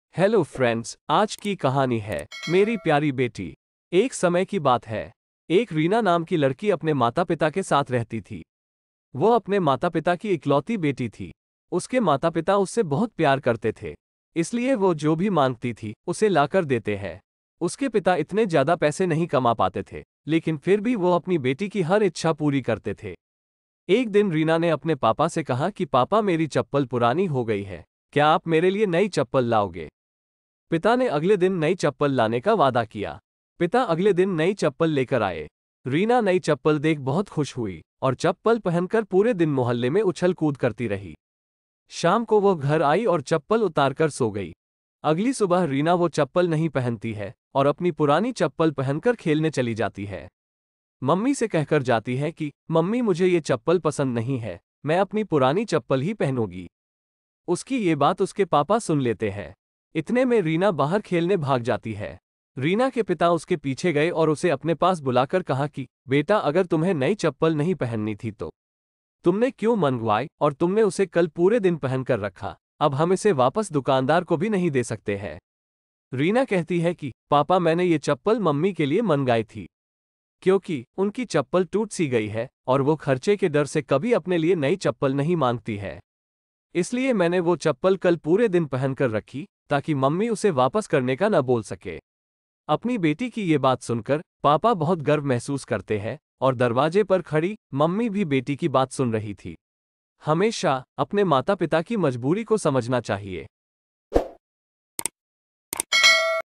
🌼 मेरी प्यारी बेटी 🌼 | हिंदी कहानियां | hearttouching | # kahani | # kahaniya | # hindi kahan
🌼 मेरी प्यारी बेटी 🌼 | hindi kahani | moralstory | kahani | hindikahniya | प्रेरक कहानी |

Namaskar  Dosto
yeh kahani ek ladki ki hai jo apne maa ke bare me sochti hai /yeh ek moral kahani hai. so please pure dekhe